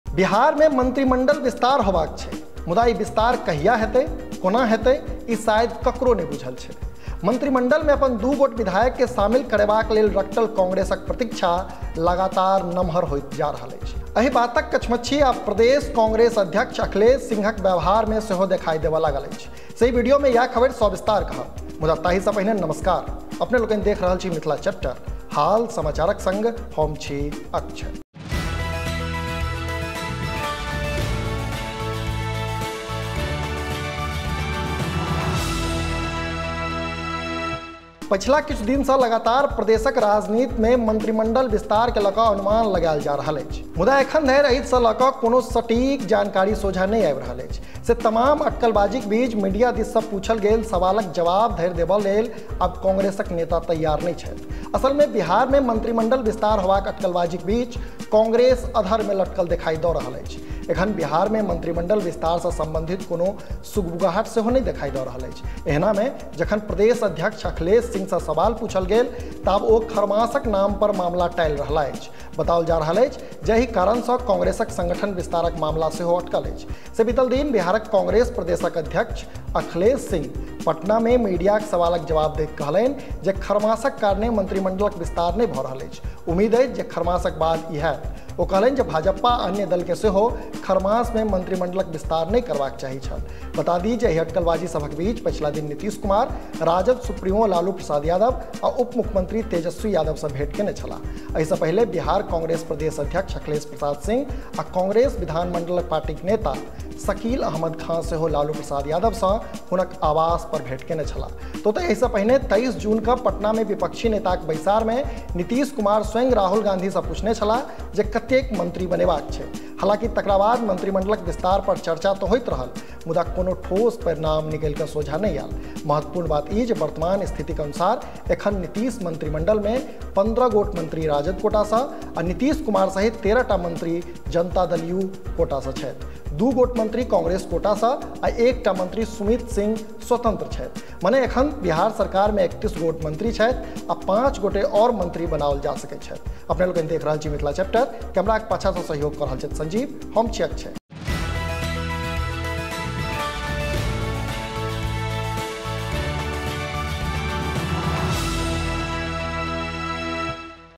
Bihar मे Cabinet विस्तारक लेल बेहाल अछि Congress, नहि द' रहलाह अछि Nitish कान-बात
बिहार मे मंत्रिमंडल विस्तार होयबाक छैक, मुदा ई विस्तार कहिया हेतैक? कोना हेतैक ? ई शायद ककरो नहि बुझल छैक। मंत्रिमंडल मे अपन दू गोट बिधायक केँ शामिल करयबाक लेल रकटल कांग्रेसक प्रतीक्षा लगातार नमहर होइत जा रहल अछि। एहि बातक कछमछी आब प्रदेश कांग्रेस अध्यक्ष अखिलेश सिंहक व्यवहार मे सेहो देखाइ देबय लागल अछि।

Host : Akshay Anand Sunny
Script : Gunjan Shree
Camera : Sanjeev Kumar 'Bittoo'
Edit : Rashi Akanksha

Thanks
Ajit Azad, (CEO, A4G Media)

Producer: Dr C M Jha